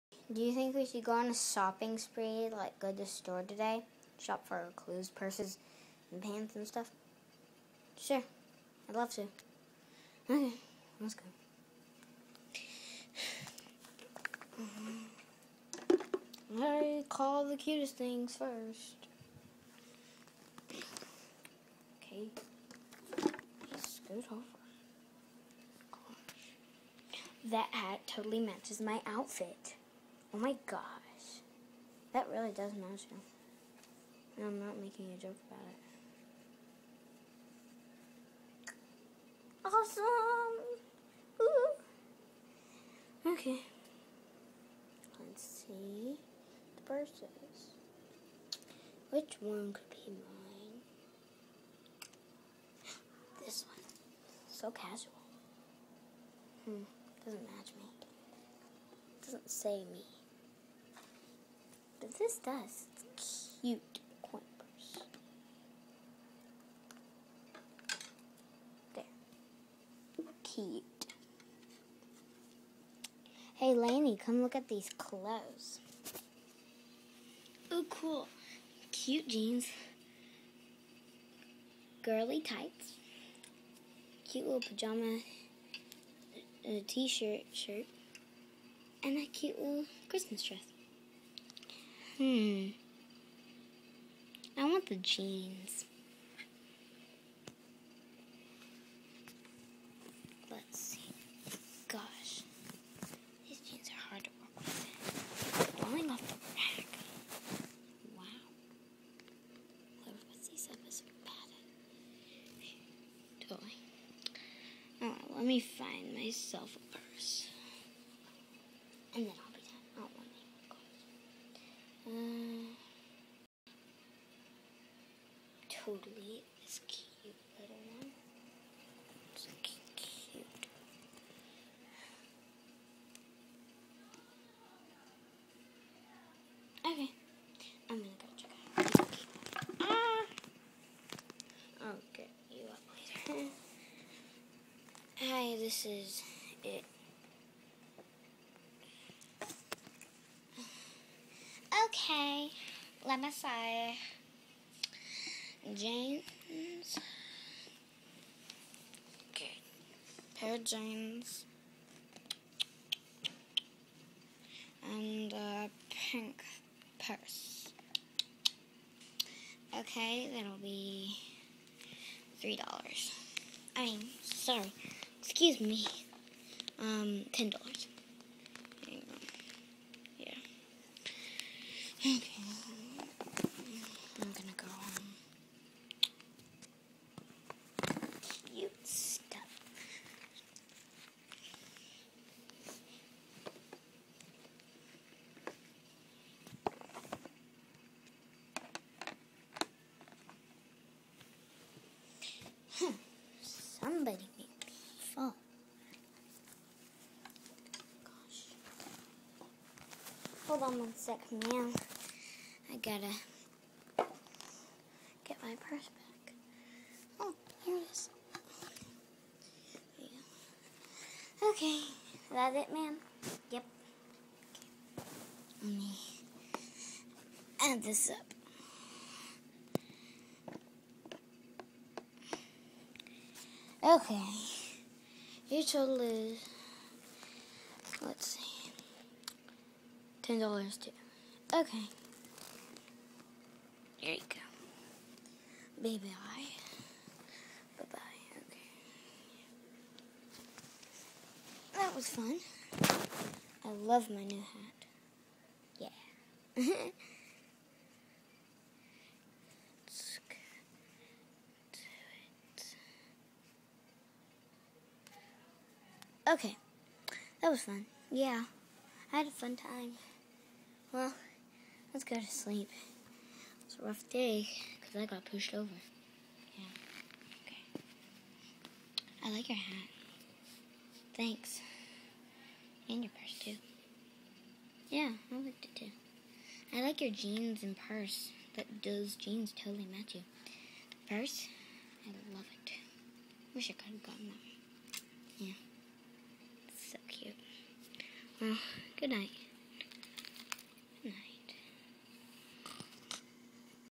AG shopping trip
Julie and Lanie go on a shopping trip.